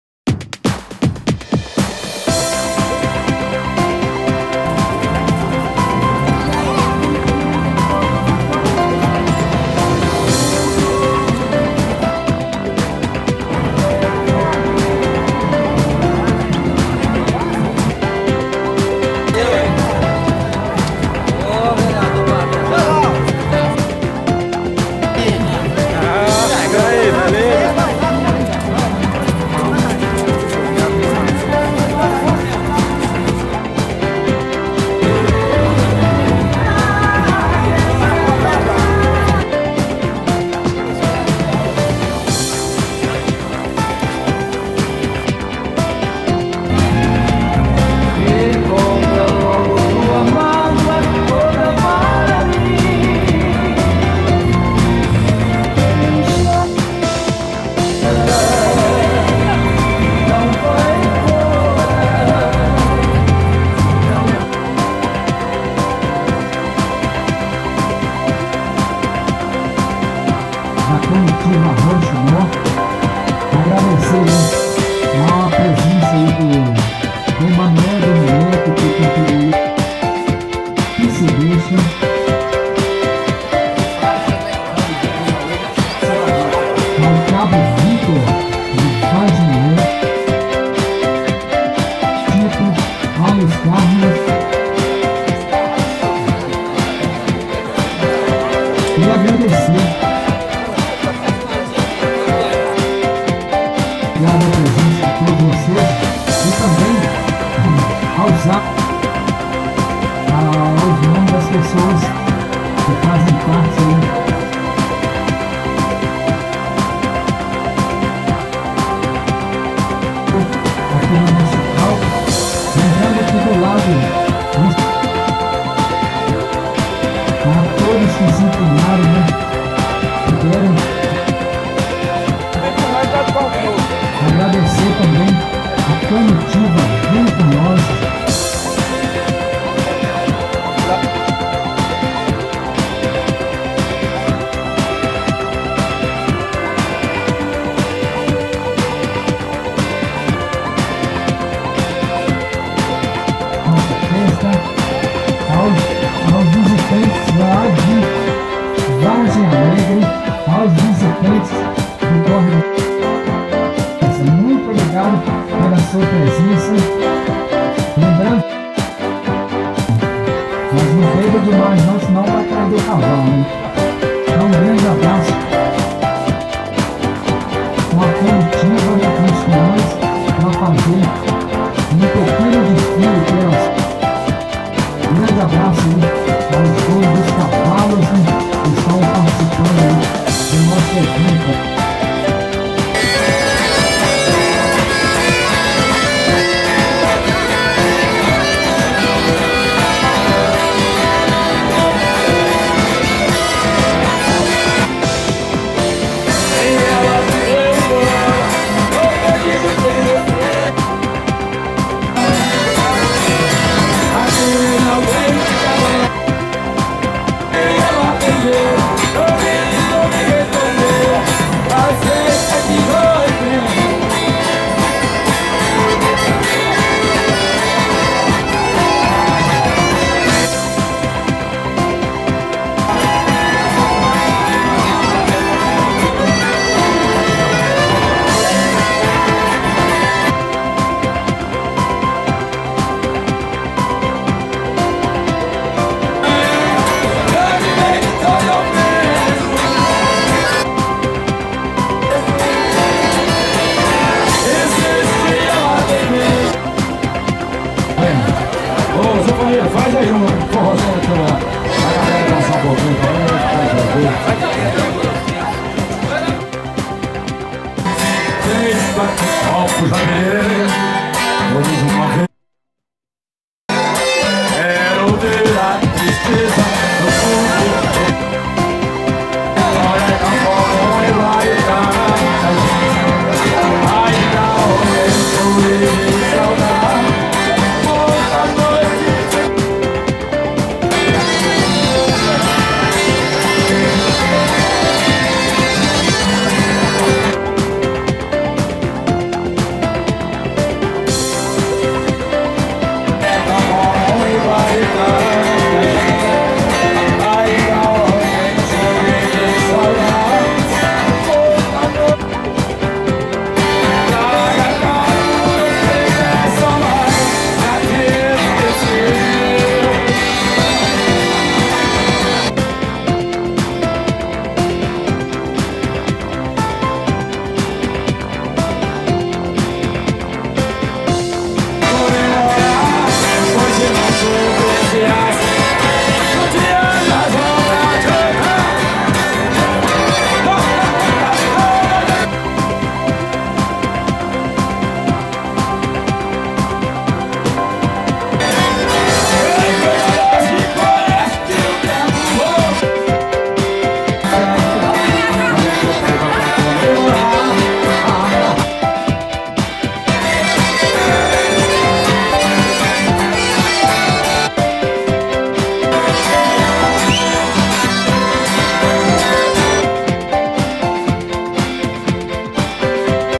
Clip - 1º- Encontro de cavaleiros da Comitiva Arrocha o nó - Pingo D'água MG
Marcelo Braga Drones e Cia:
Domingo 06 de maio de 2018 - segundo dia de apresentação do Circo em Pingo D'água MG